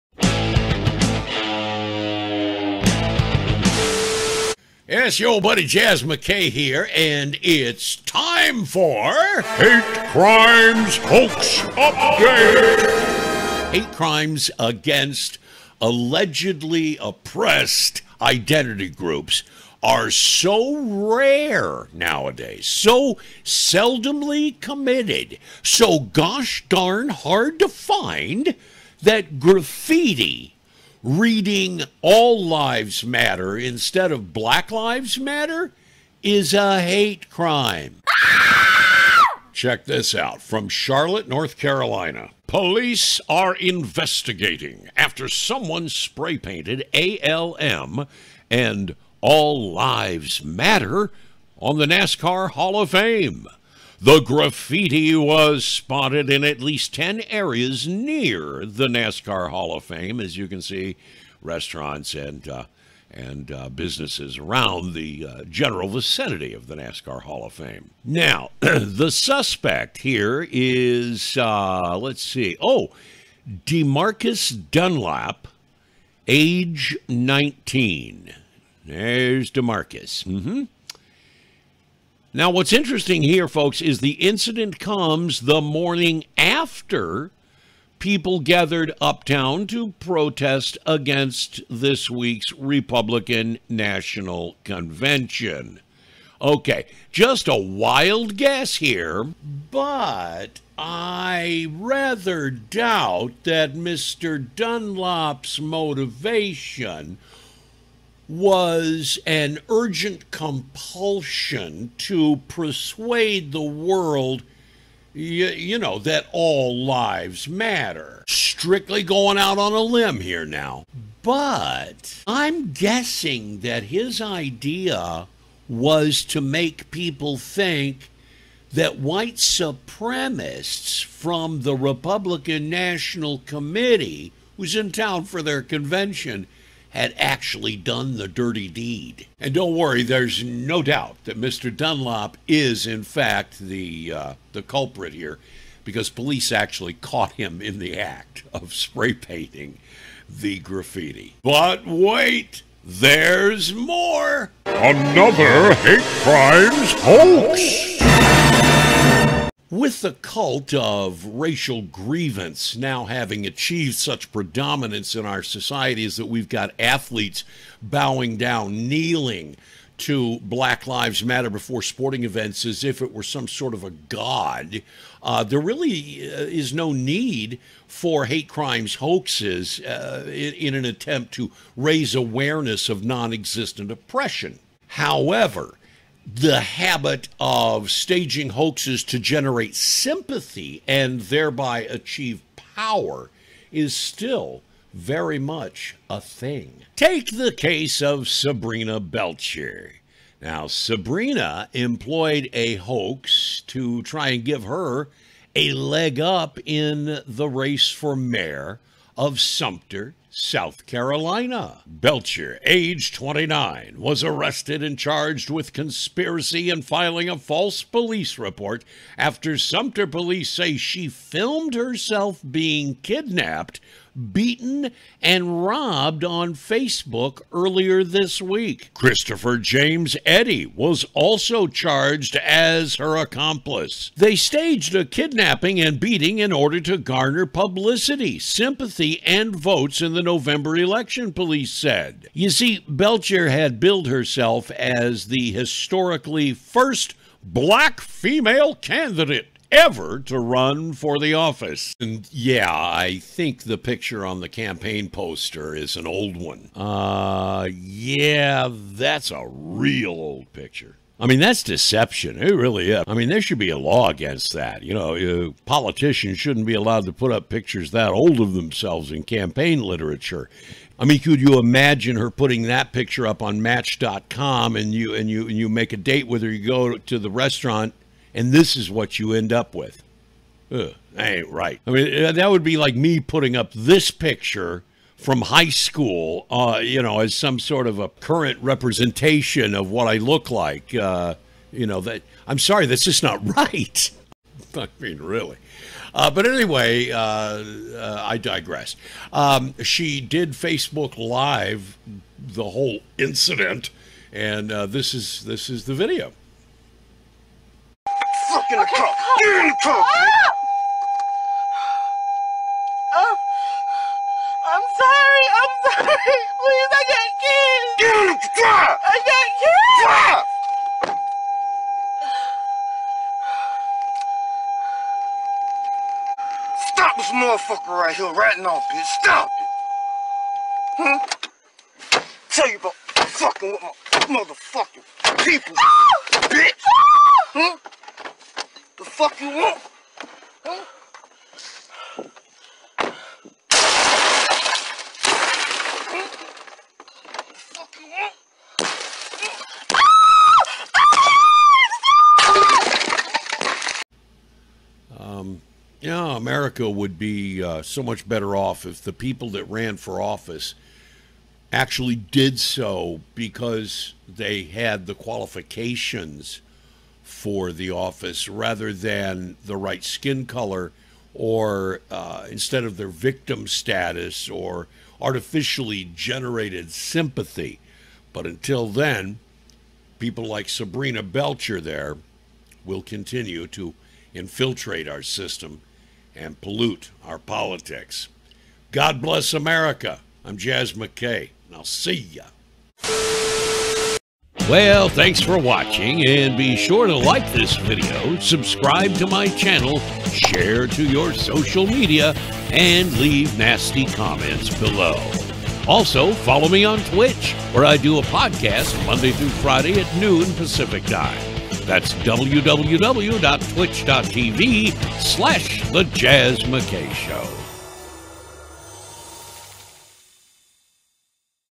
A HATE CRIMES HOAX UPDATE
Hate crimes are so rare they actually have to be invented out of thin air.